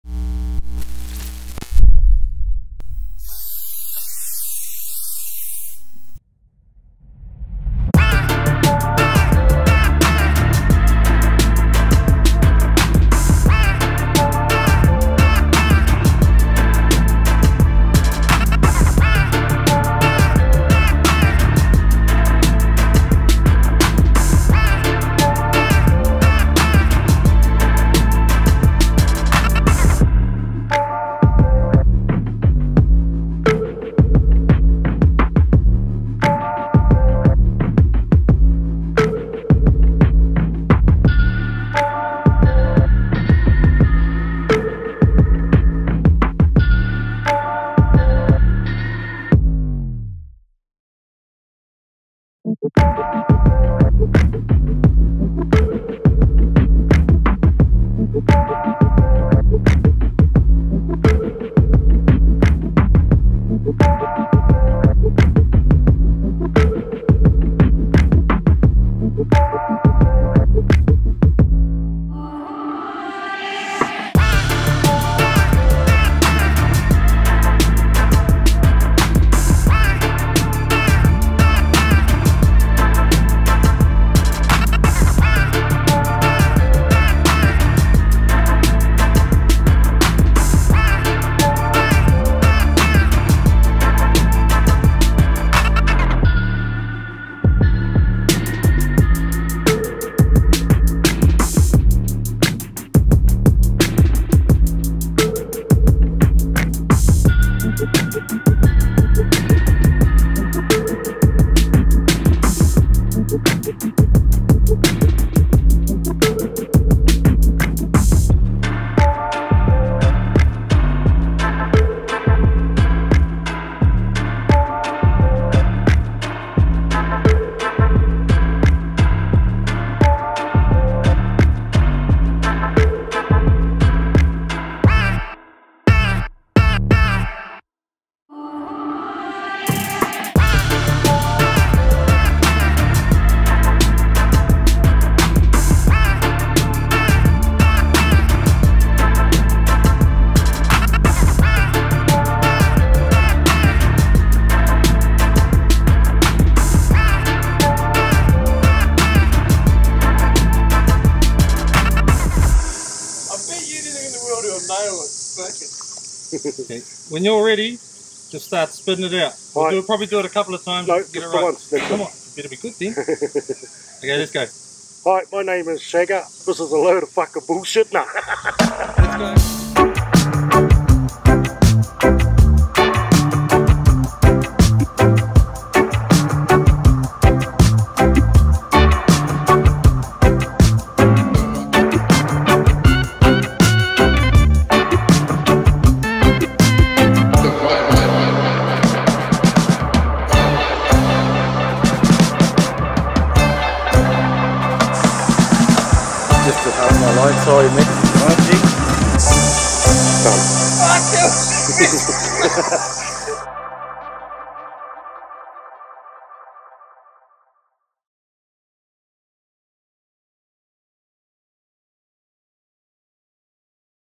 Loggabull extra scenes video
Quick edit of extra footage and bloopers from the day of shooting at crew 14